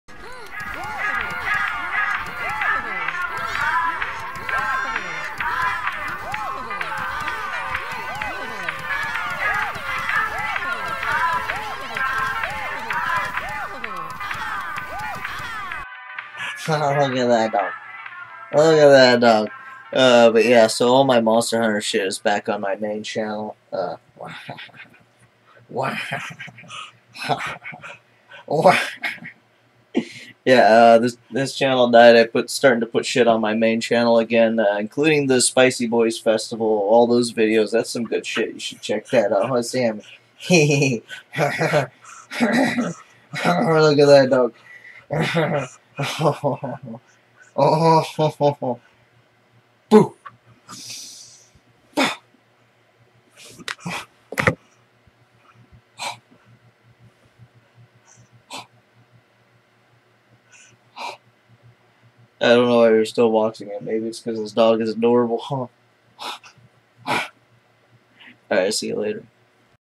where did all the videos go? back to my main channel
elden ring came out a week or two after my friends convinced me to make this channel. anyways so I went back to uploading clips on my main channel but never posted anything over here so, here you go.